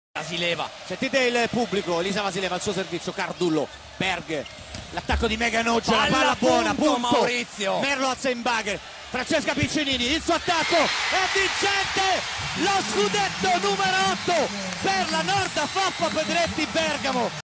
Bergamo 8 volte Campione d'Italia: Ultimo punto di Piccinini
Play Off 2011: Finale: Gara 5: Villa Cortese-Bergamo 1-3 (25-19, 22-25, 24-26, 20-25).
Al termine di un match fantastico la Norda Foppapedretti Bergamo si laurea Campione d'Italia per l'ottava volta nella sua storia